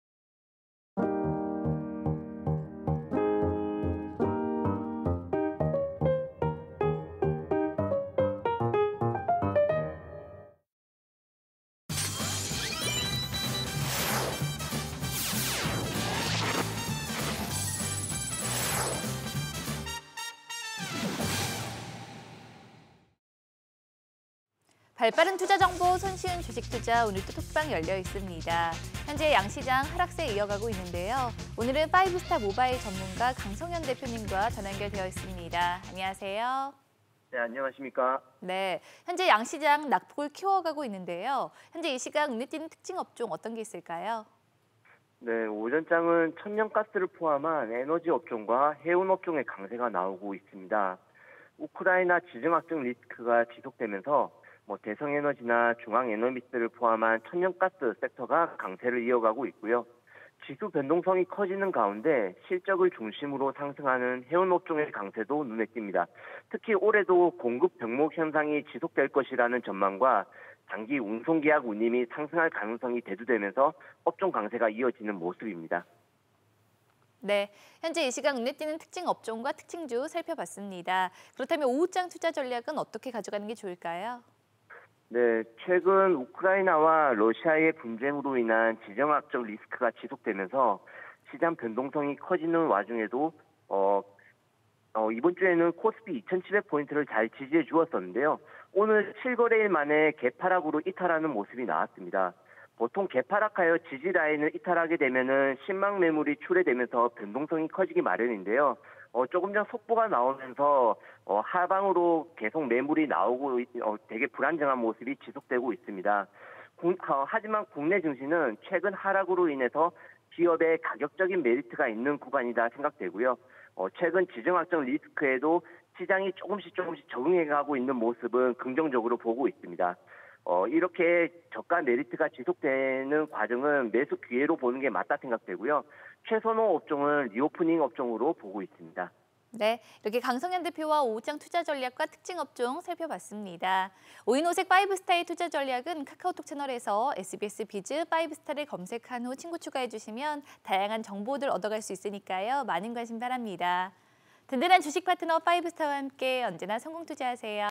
🛫우크라 이슈 지속❗ 강세보이는 천연가스 섹터의 '대성에너지'
오늘 오후장 및 내일장 투자전략 /뉴스타투자전략

SBSBiz 모바일 주식정보 '뉴스타' 서비스의 투자전략을 소개해드립니다!

■ 5스타, 필살기 등 SBS Biz의 고급진 콘텐츠는 'SBS Biz 증권' 채널 구독!

■ 우리집 가계부 재무관리, 보험료는 줄이고 보장은 높일 콘텐츠는 '보통의 돈 이야기'를 검색하세요!